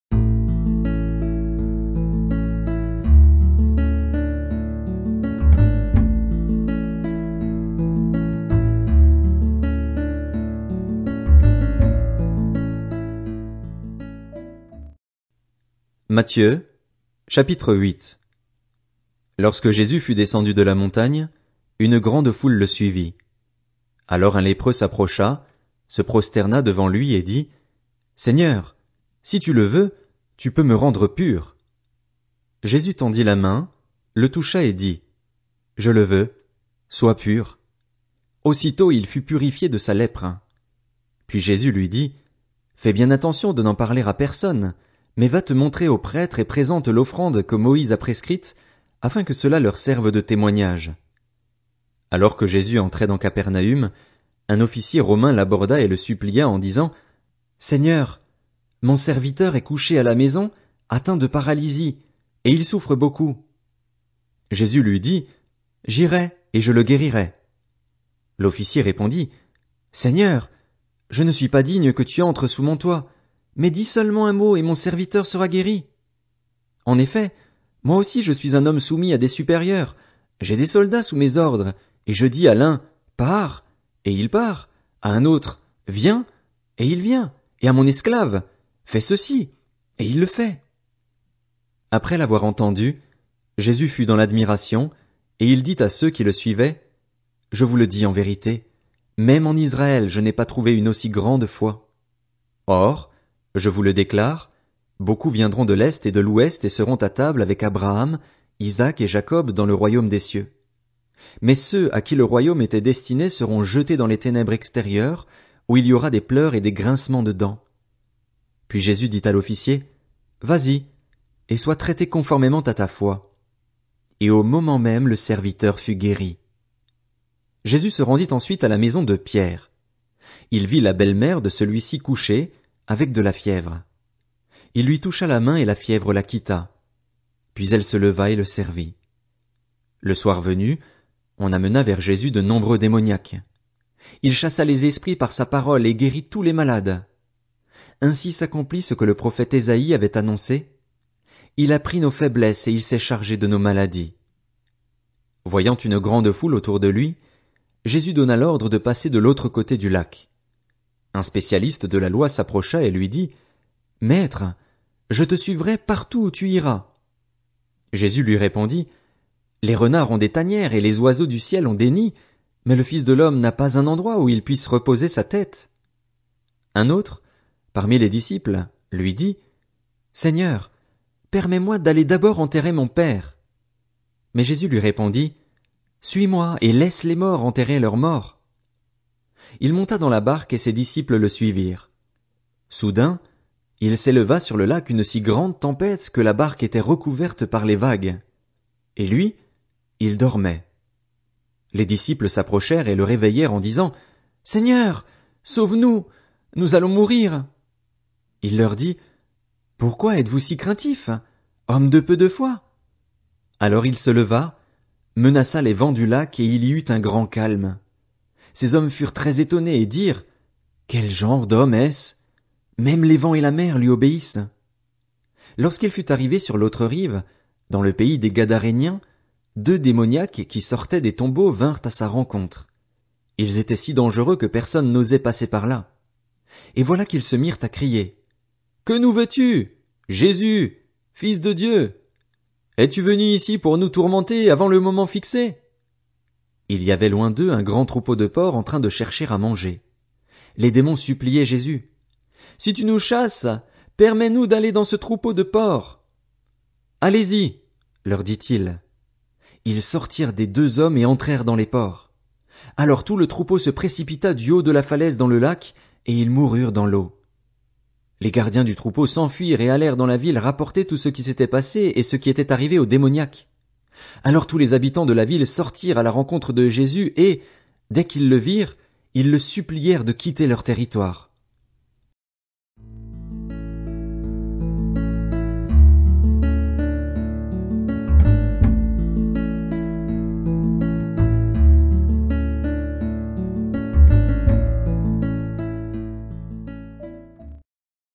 Écoute des évangiles du 5 Juillet
Votre rendez-vous quotidien avec la Bible. L'écoute du jour est tirée du livre de Matthieu, le chapitre 8. Bonne écoute!